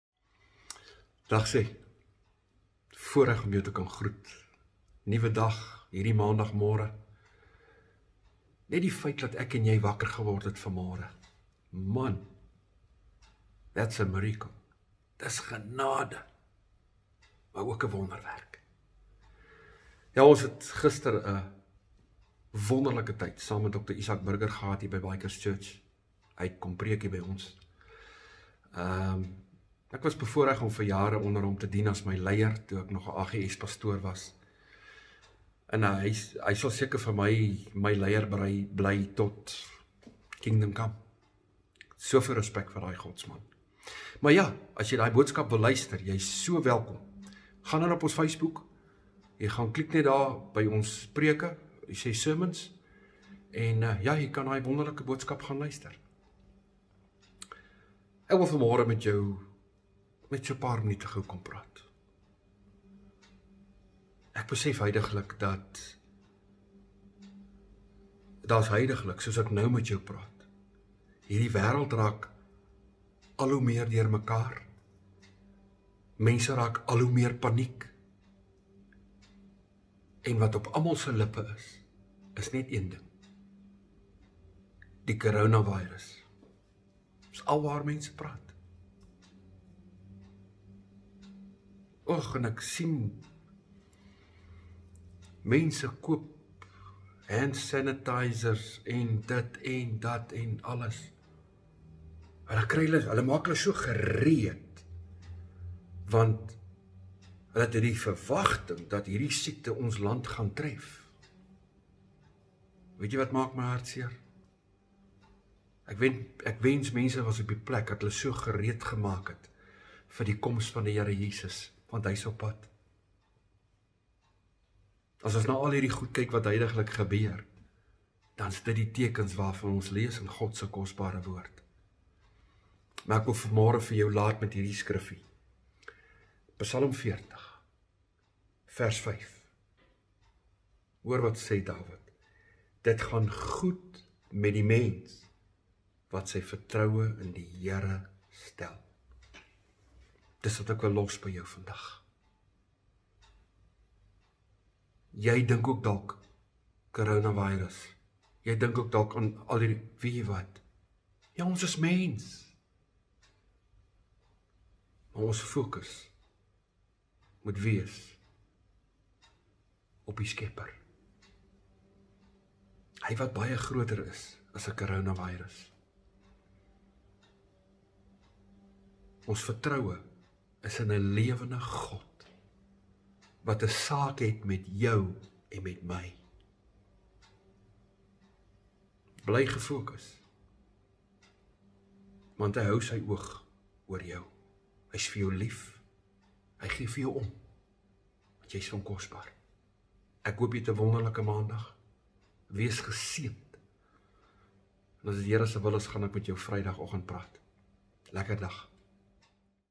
Fokus op die lewende God - Past Ben Vorster
Psalm 40:5
Dit gaan goed met die mens wat sy vertroue in die Here stel
en hom nie wend tot duister magte
en tot mense wat hulle met skyngode ophou nie.